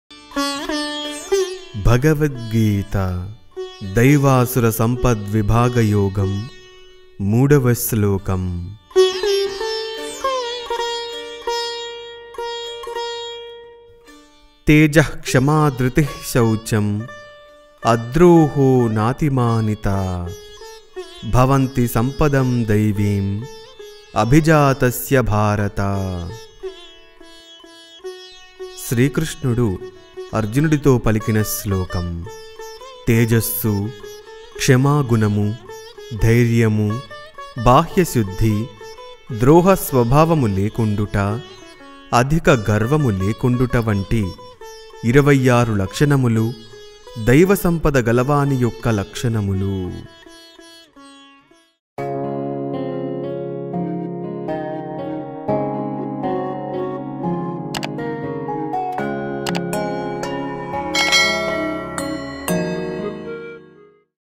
Bhagavad Gita Telugu | Daivasura Sampad Vibhagayoga | Chapter 16 - Verse 3 || TeluguTraditions
అసుర లక్షణములు, దైవ లక్షణములకు మధ్య అంతరమును భగవంతుడు వివరించెను. మానవులు మనుష్యులుగా, మానవత్వముతో జీవనం సాగించుటకు ఏ లక్షణములను అలవరచుకోవాలి, ఏ లక్షణములకు దూరముగా వుండాలి అనే విషయములను తెలుసుకొనుటకు ఈ అధ్యాయము ఎంతగానో ఉపయోగపడుతుంది.

దైవీ భావములు గల వారిలో ఏ గుణములు ప్రస్ఫుటిస్తాయి అలాగే అసురీ భావములు గలవారిలో ఏ లక్షణములు ప్రస్ఫుటిస్తాయి అనే విషయములో ఆ భగవానుడు ఎంతో విపులముగా తెలియ చేసారు. కనుక ఈ అధ్యాయము ప్రతి ఒక్కరికి ఆచరణాత్మకమైన జ్ఞానమును ప్రసాదిస్తుంది.